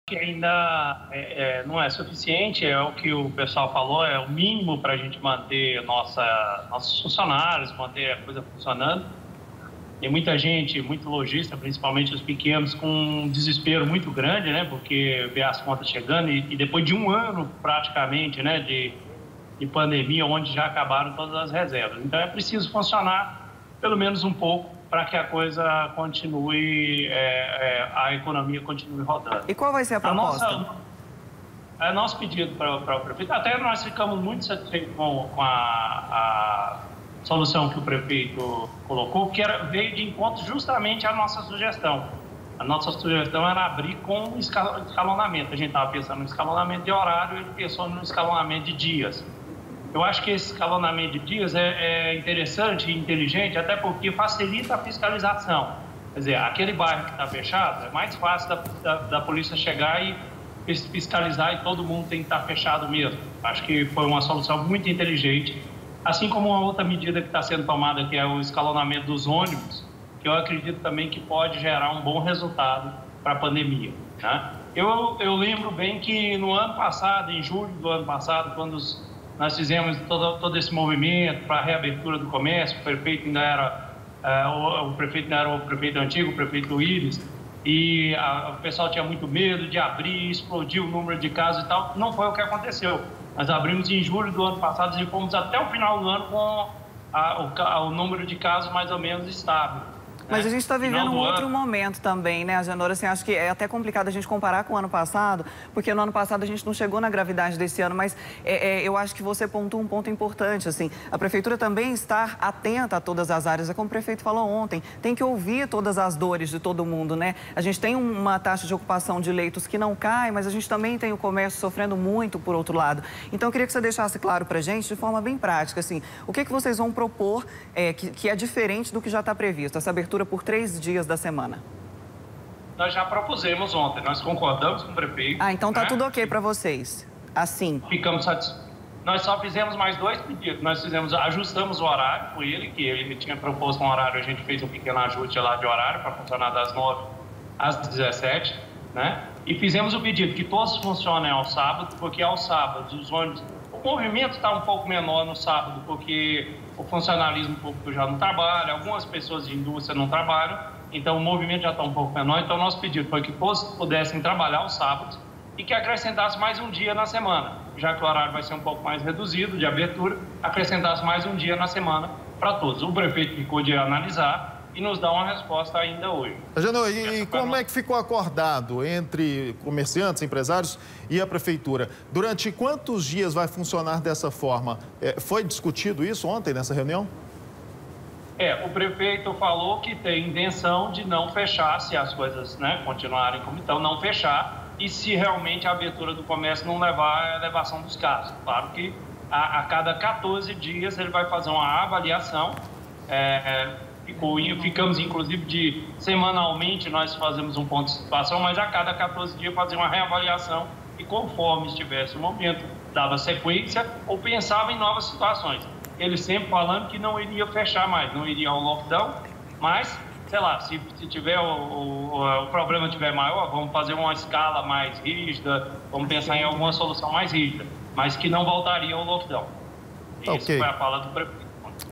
ESCALONAMENTO: EMPRESÁRIOS QUEREM QUATRO DIAS DE ABERTURA
ESSE DECRETO QUE DEVE SAIR AINDA HOJE NASCEU DE VÁRIAS REUNIÕES DO PREFEITO COM OS SEGMENTOS COMO ACIEG, ASSOCIAÇÃO DA 44, ABRASEL. MAS NEM TUDO ESTÁ DENTRO DO ESPERADO. UMA DAS PROPOSTAS DOS SEGMENTOS É QUE OS ESTABELECIMENTOS POSSAM ABRIR 4 DIAS AO INVÉS DE TRÊS E TAMBÉM AOS SÁBADOS. VAMOS FALAR COM O AGENOR BRAGA QUE É CONSELHEIRO DA CÂMARA DE DIRIGENTES LOJISTAS.